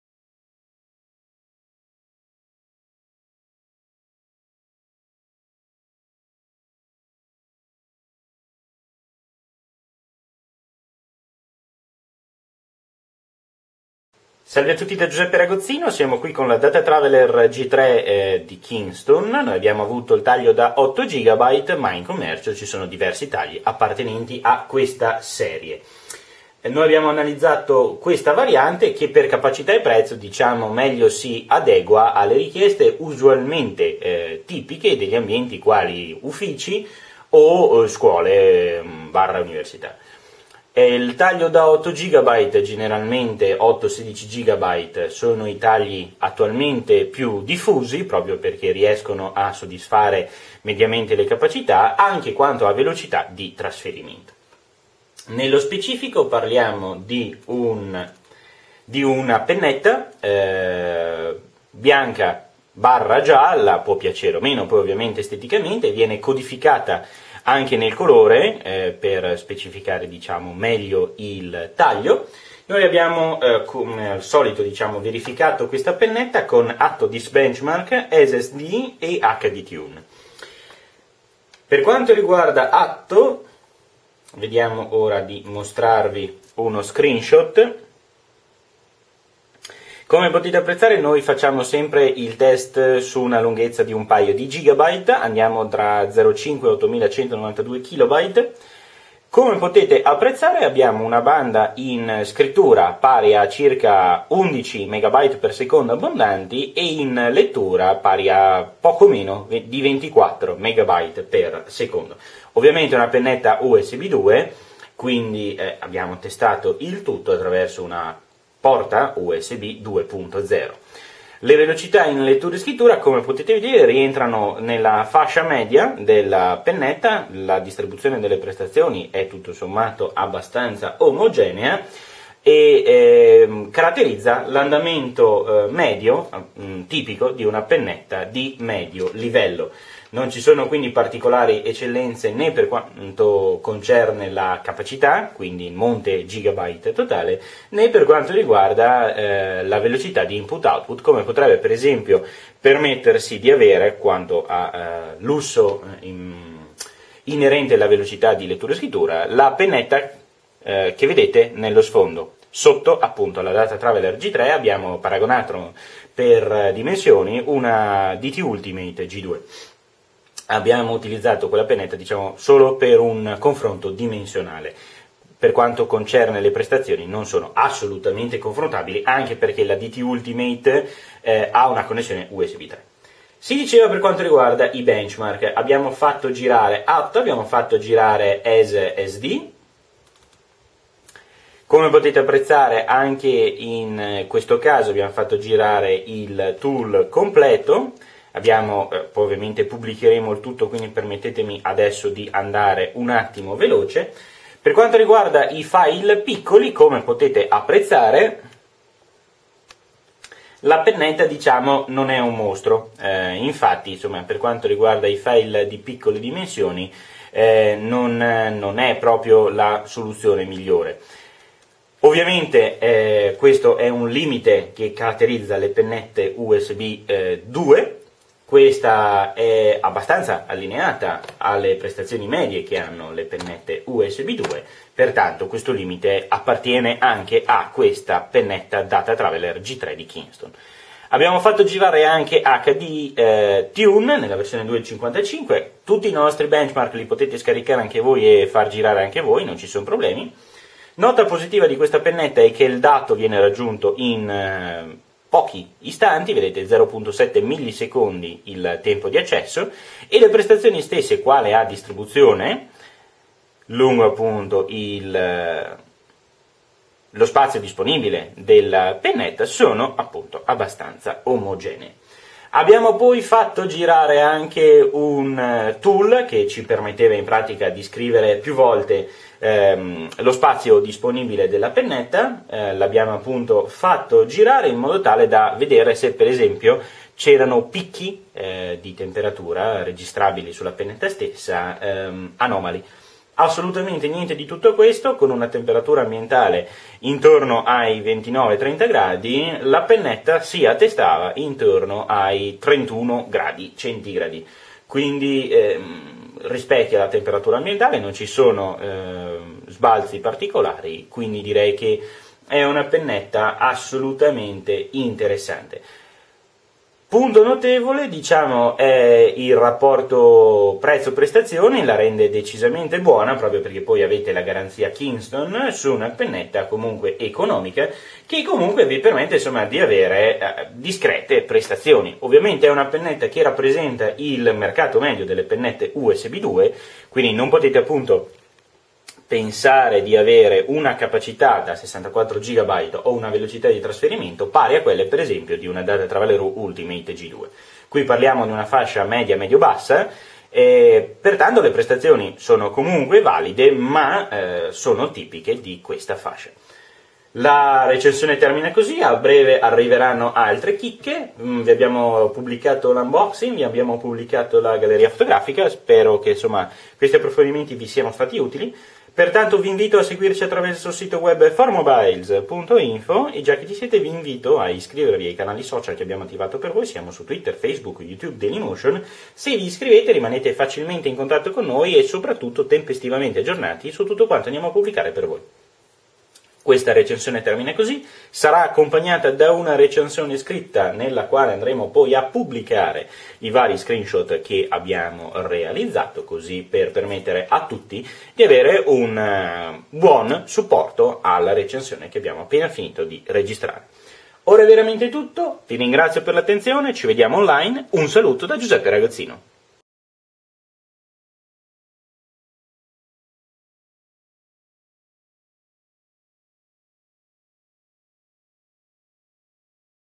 Breve recensione su Kingston DataTraveler G3 (8 GB)
Eccoci in un rapido passaggio video dove riassumiamo le nostre più importanti considerazioni in merito a questa pendrive USB 3 da 8 GB, elemento corrispondente alla fascia mediobassa della gamma Kingston inerente le penne digitali USB 2 di questo termine 2011.
Il device non è dotato di eccellenze particolari quanto a velocità di trasferimento dati e così, prima d'addentrarci in analisi più approfondite, mi sembrava interessante mostrarvi un sunto del nostro pensiero su questa penna nata per essere il tradizionale muletto da ufficio/scuola.